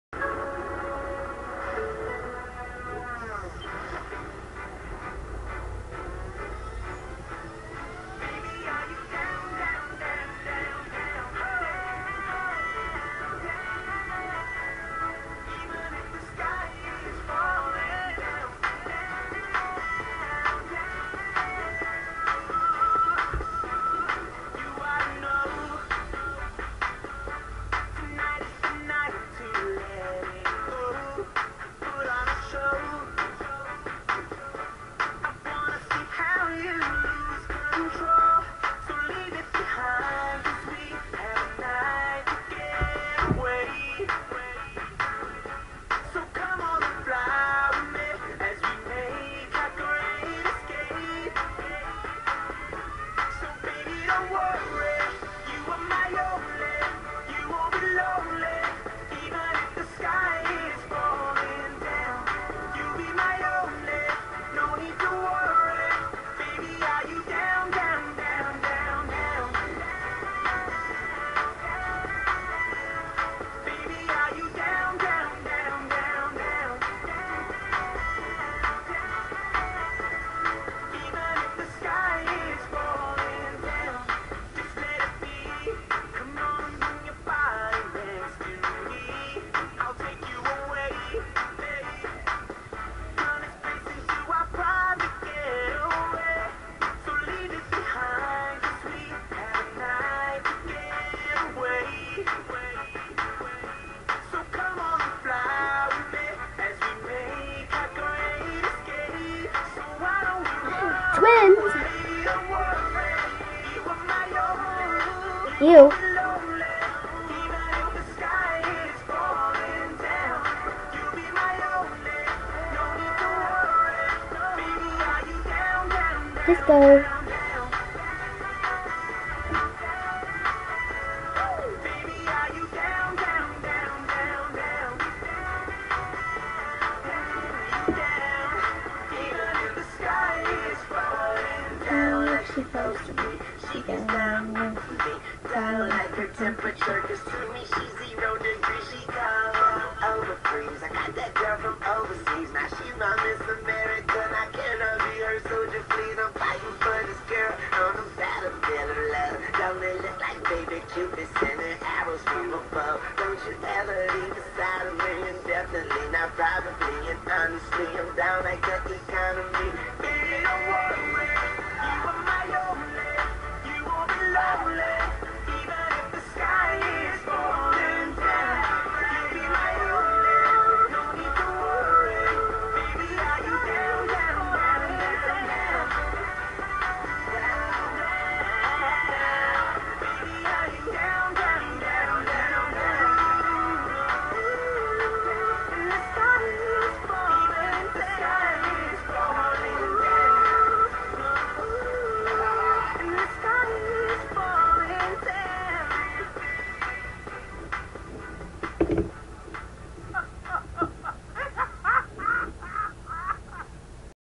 PICS
ITS PICS OF ME AND MY MOM AND THE AVTARS SINGING DOWN LOL :}